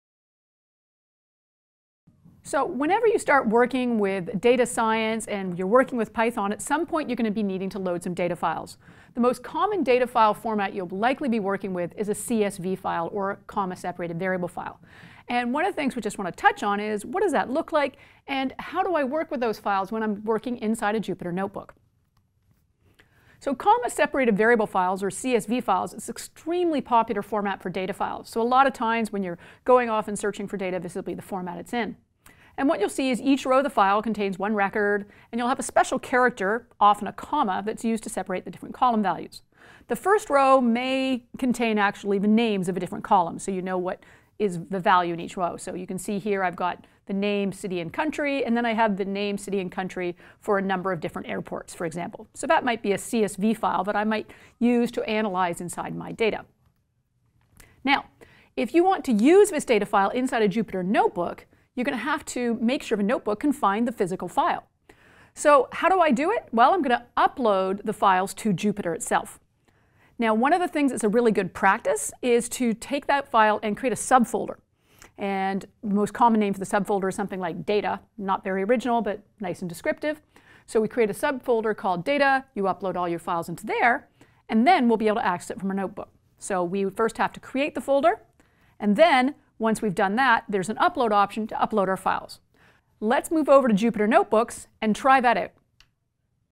CSV files and Jupyter | Even More Python for Beginners - Data Tools [11 of 31]
The first step to working with data is, as one might expect, getting the data into the location in which you'll be working with it. We'll chat about the types of data files you'll most commonly work with, and how to upload those into your Jupyter instance.